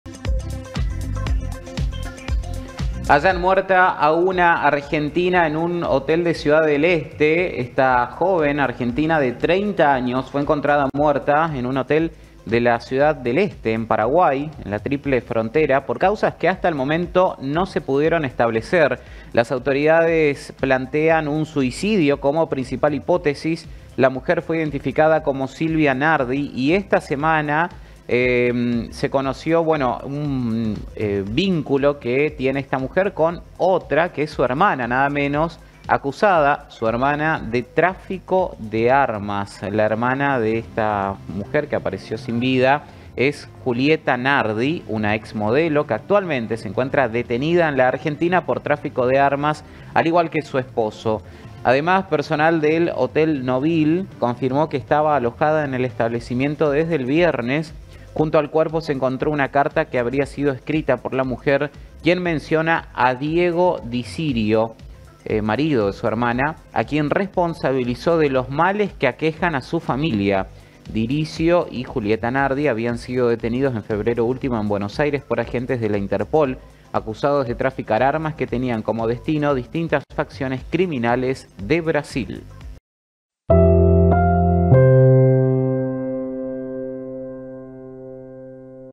NGFEDERAL - HALLAN MUERTA A UNA ARGENTINA EN HOTEL DE PARAGUAY
Una joven argentina de 30 años fue encontrada muerta en un hotel de Ciudad del Este, en Paraguay, por causas que hasta el momento no se pudieron establecer. Las autoridades plantean un suicidio como principal hipótesis.
La mujer fue identificada como Silvia Nardi y era hermana de Julieta Nardi, una ex modelo que actualmente se encuentra detenida en la Argentina por tráfico de armas, al igual que su esposo. Además, personal del Hotel Nobile confirmó que estaba alojada en el establecimiento desde el viernes pasado.
Junto al cuerpo se encontró una carta que habría sido escrita por la mujer, quien menciona a Diego Dirisio -marido de su hermana-, a quien responsabilizó de los males que aquejan a su familia.
Dirisio y Julieta Nardi habían sido detenidos en febrero último en Buenos Aires por agentes de Interpol, acusados de tráficar armas que tenían como destino a distintas facciones criminales de Brasil.